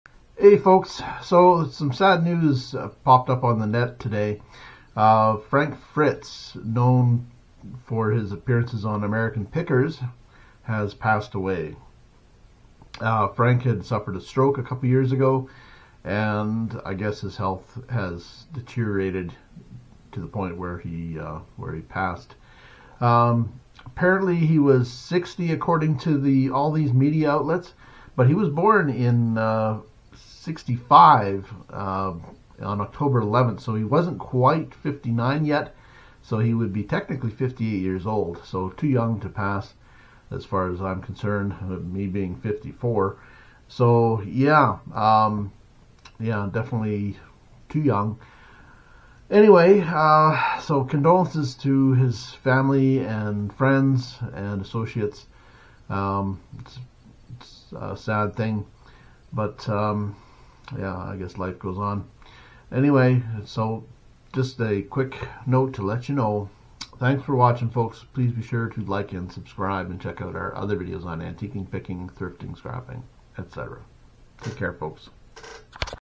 TRAGIC News Re: FRANK FRITZ - Media Outlets Got It WRONG
Frank Fritz of American Pickers fame has passed away. but Media Outlets got a detail very wrong...